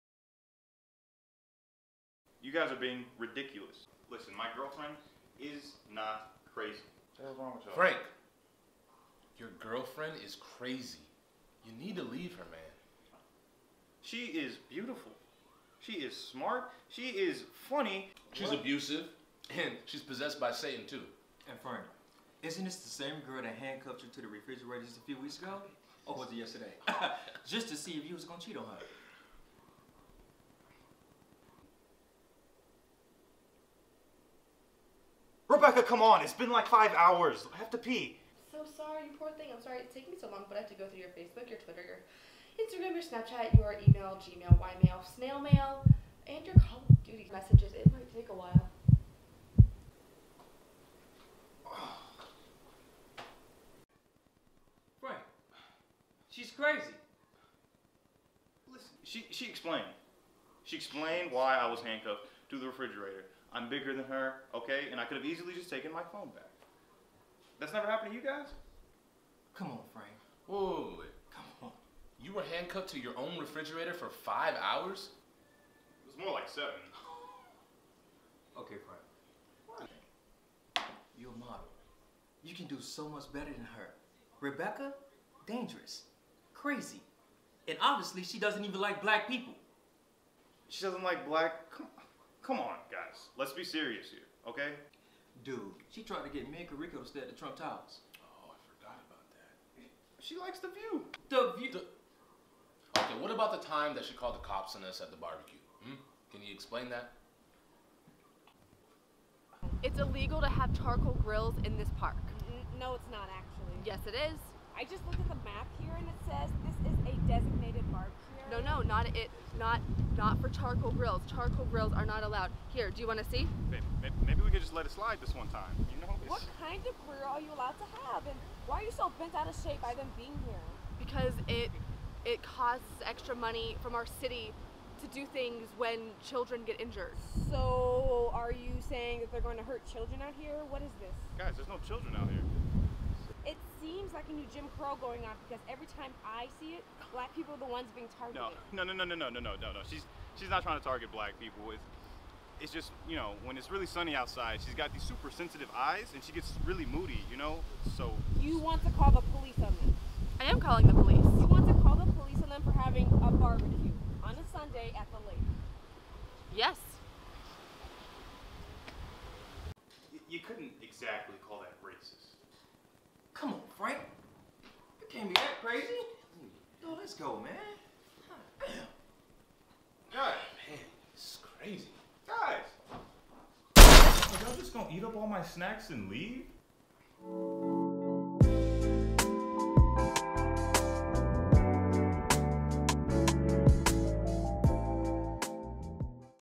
The Oblivious Boyfriend Frank Smith (Comedy)
Should I break Rebecca’s heart or should we let our strange love prevail? Check out this hilarious comedy scene including a Paradoy Scene of viral sensation “BBQ Becky” produced and written for Phaedra’s “Create a Character” challenge. Enjoy!